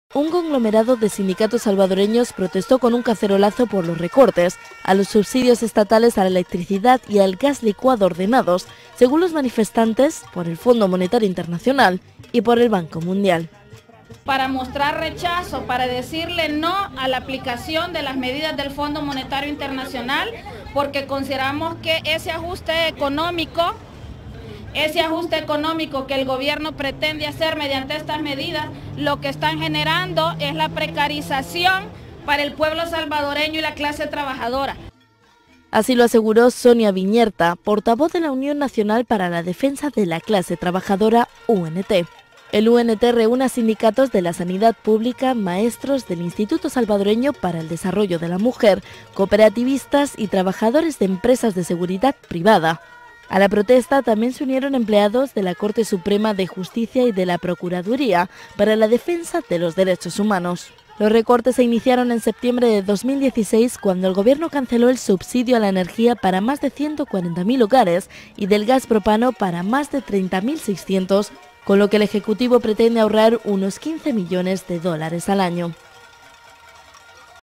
El Salvador: Cacerolazos contra recortes a los subsidios a la electricidad y al gas licuado
Sindicatos en El Salvador protestaron con cacerolazos en contra de los recortes a los subsidios a la electricidad y al gas licuado.

Redacción CB24 / Agencia EFE